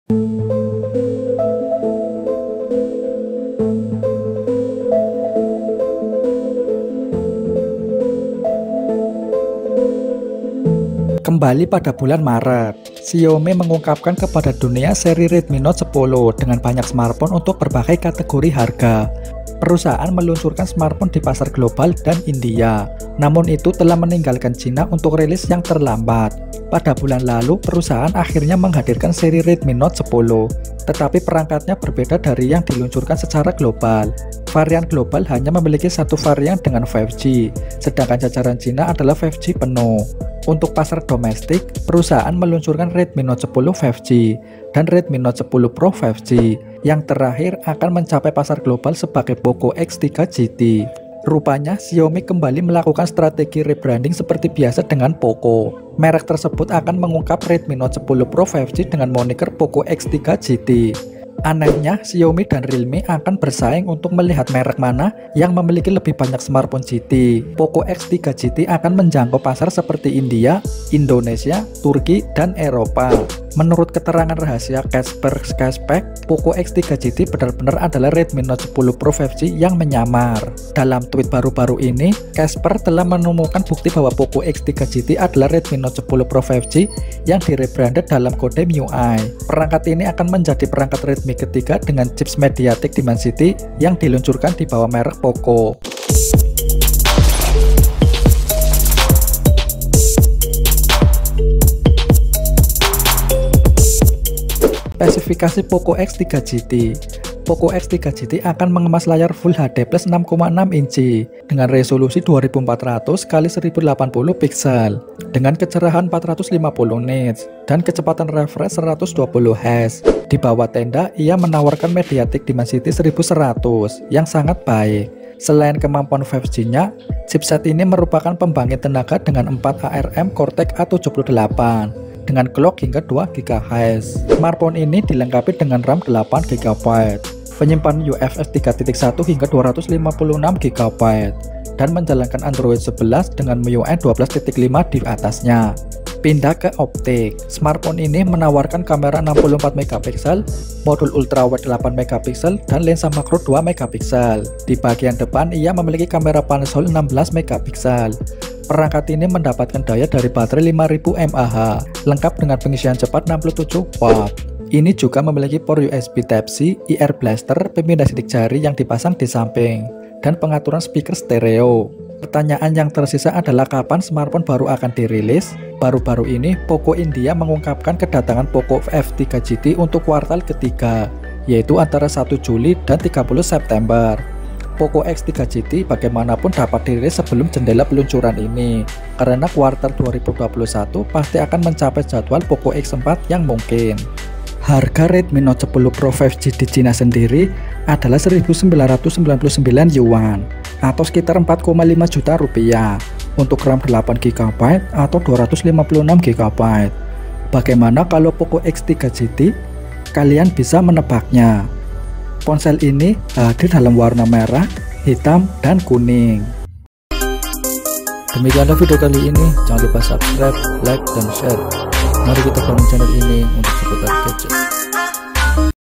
POCO X3 GT (5G) HARGA DAN SPESIFIKASI KAPAN RILIS DI INDONESIA ?
poco x3 gt harga dan spesifikasi, kapan dirilis di indonesia ? baru-baru ini, keterangan rahasia, Kacper Skrzypek telah menemukan bukti bahwa hp POCO X3 GT adalah Redmi Note 10 Pro 5G yang di-rebrand dalam kode MIUI. score antutu poco x3 gt adalah 438115 spek poco x3 gt sangat kuat buat test gaming, harga poco x3 gt 5g tak jauh berbeda sama redmi note 10 pro. POCO X3 GT akan menjangkau pasar seperti India, Indonesia, Turki, dan Eropa